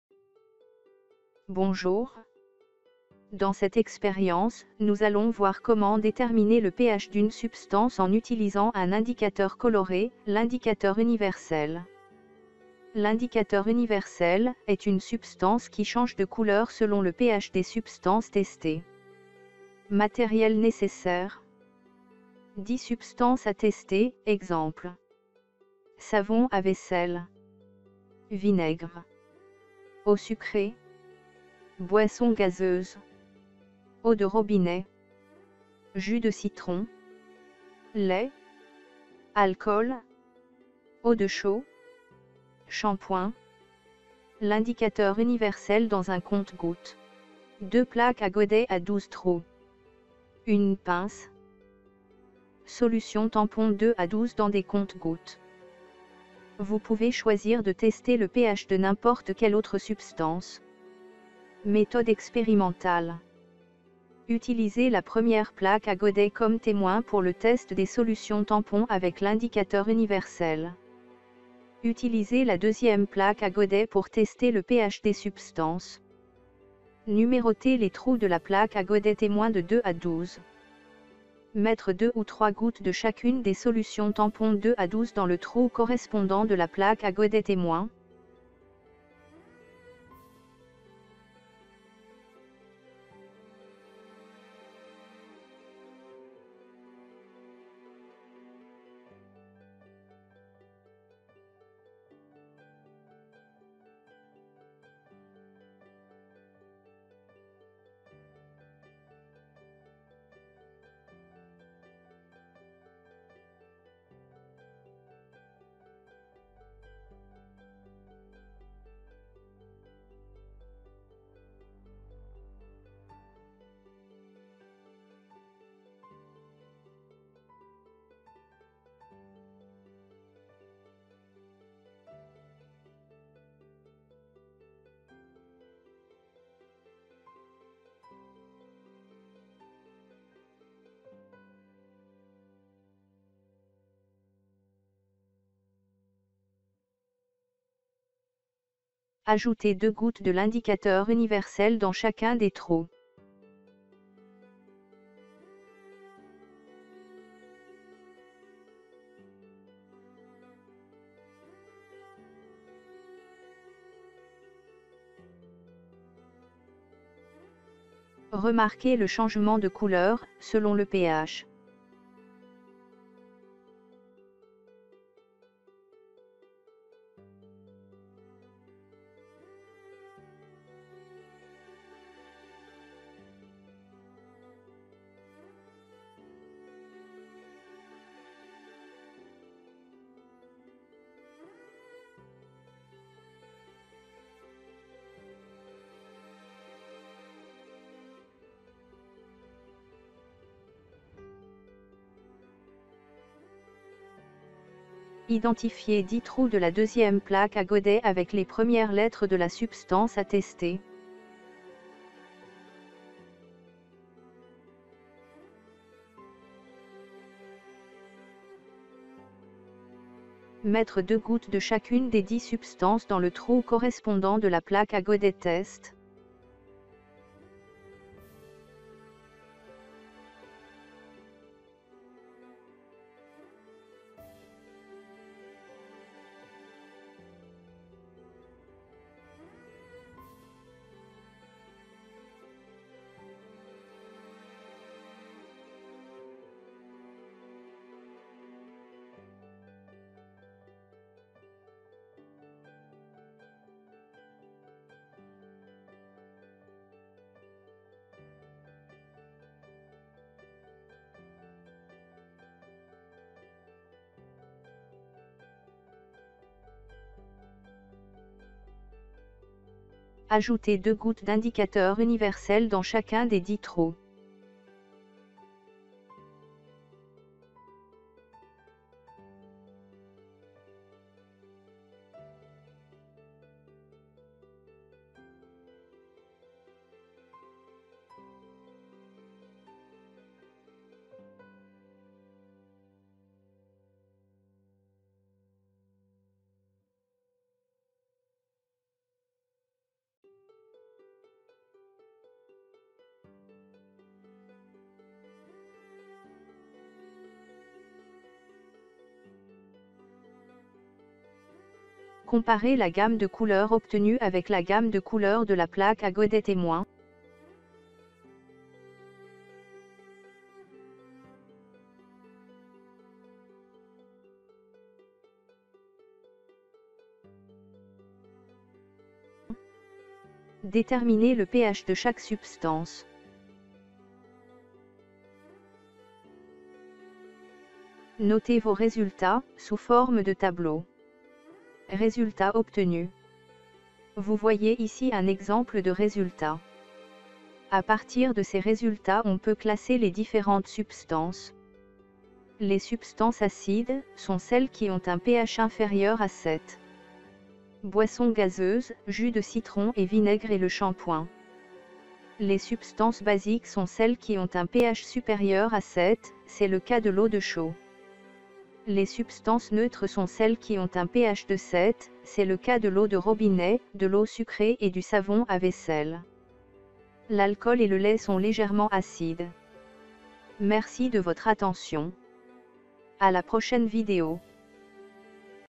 COMMENT DÉTERMINER LE PH D'UNE SUBSTANCE AVEC L'INDICATEUR UNIVERSEL?
Dans cette expérience, nous allons voir comment déterminer le PH d’une substance en utilisant un indicateur coloré, l’indicateur universel.
L’indicateur universel, est une substance qui change de couleur selon le PH des substances testées.